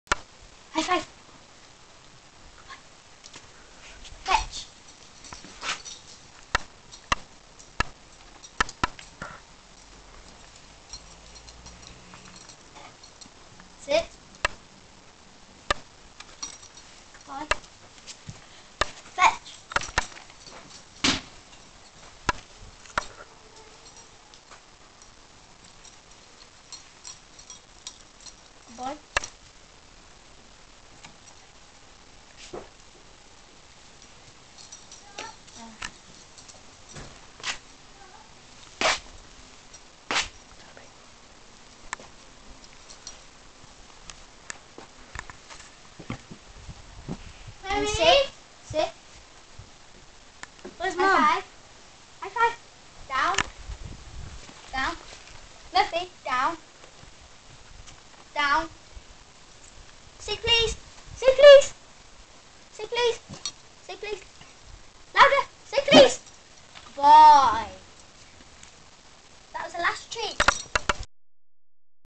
Fetch With Murphy The Sprocker
Fetch with my dog murphy in garden